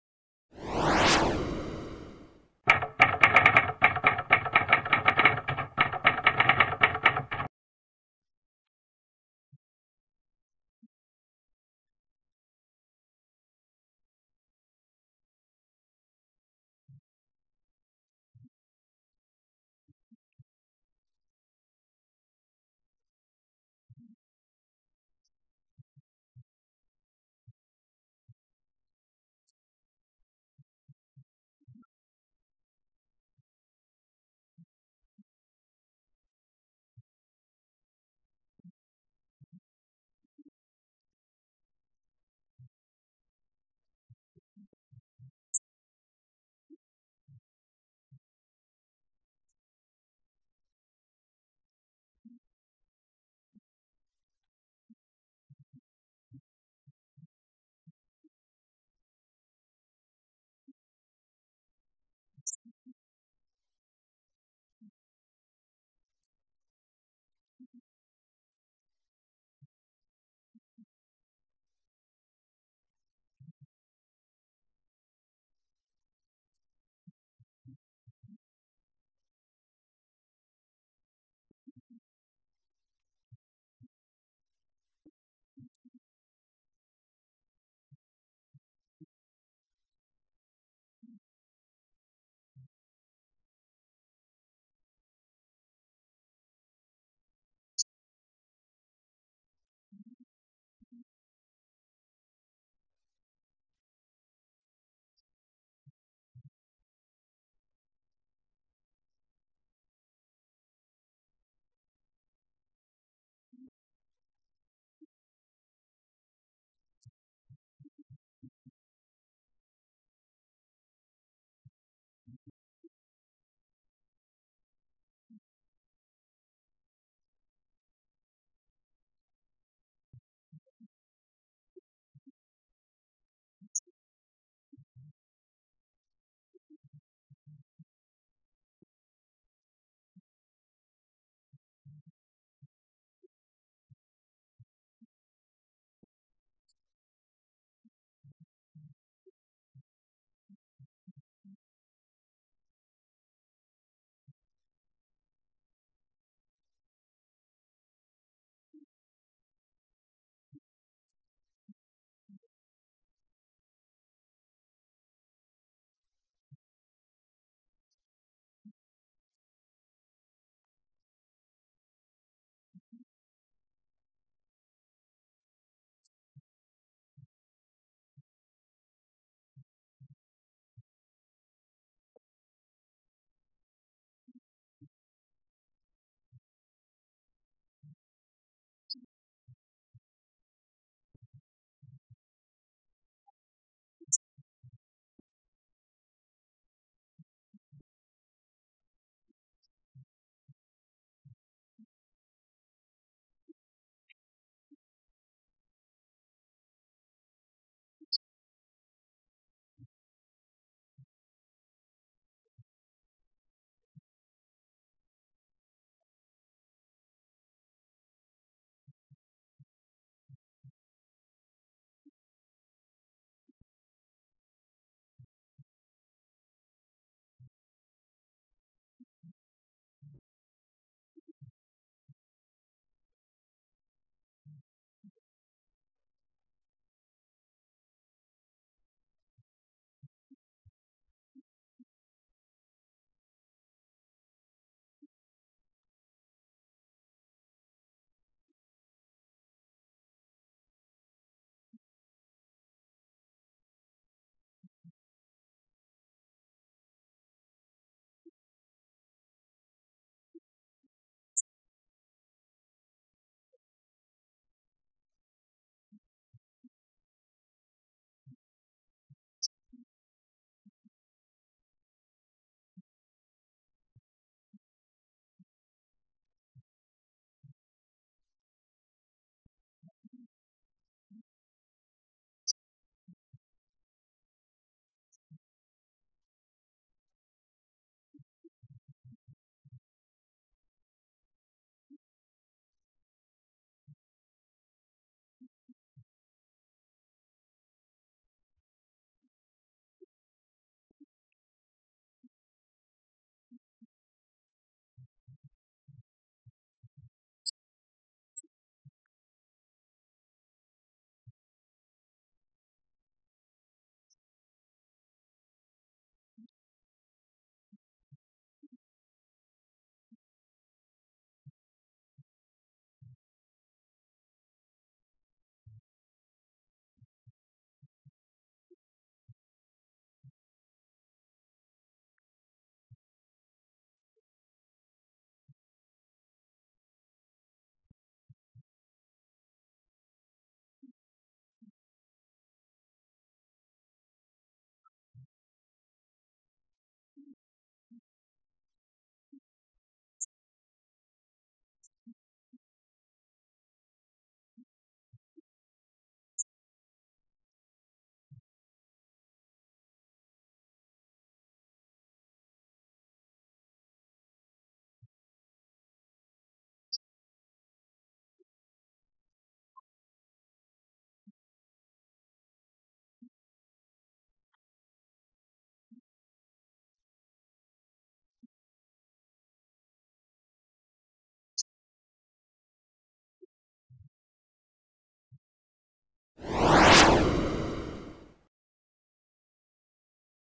REVISTA INFORMATIVA DEMOCRACIA: DEUDA ESTADO - IESS
El constitucionalista, Santiago Guarderas, realiza un análisis sobre la situación entre el Estado Ecuatoriano y el Instituto Ecuatoriano de Seguridad Social.